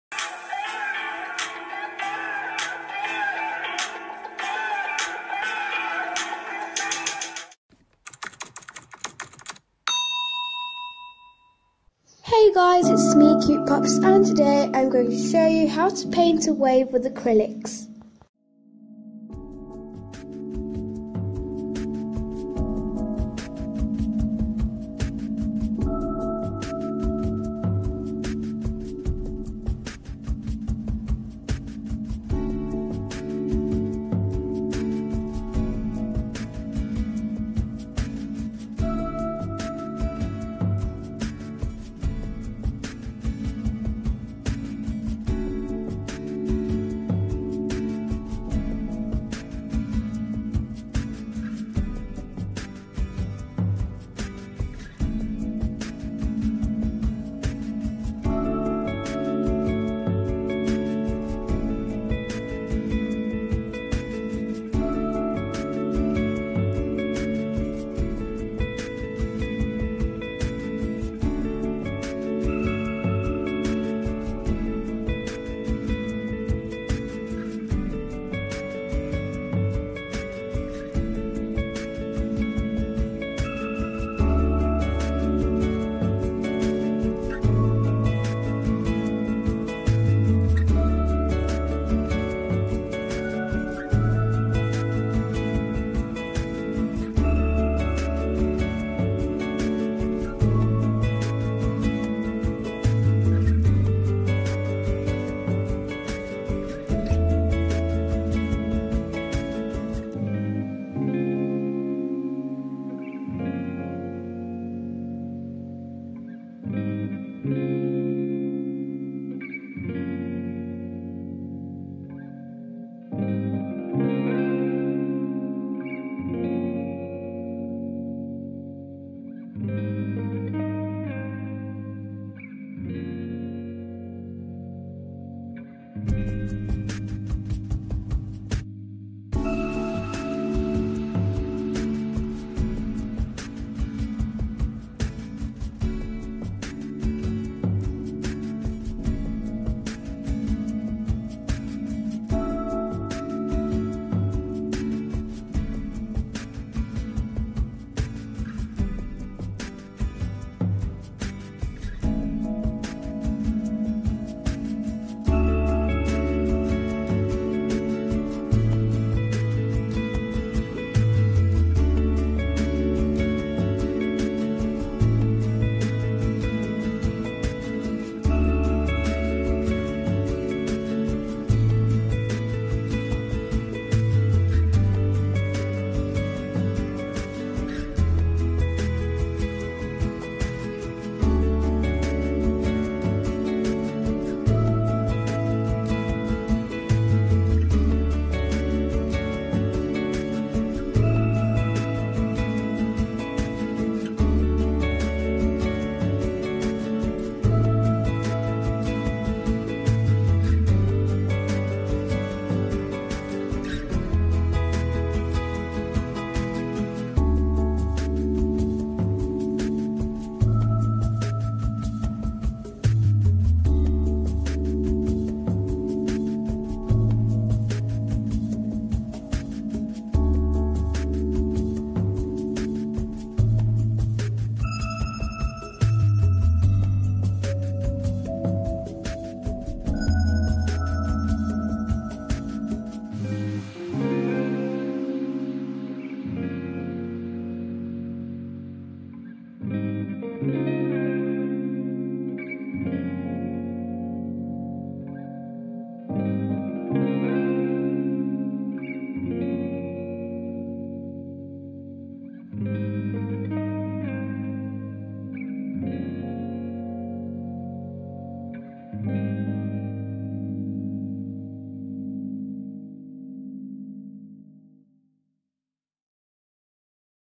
How to paint a WAVE with ACRYLICS
Hey guys! Sorry for the spelling error on the thumbnail...
This week I decided to paint an acrylic wave. It took me lots of work, but I thing the final result is really cute. I hope you like it and if you do, don't forget to smash the like botton! :)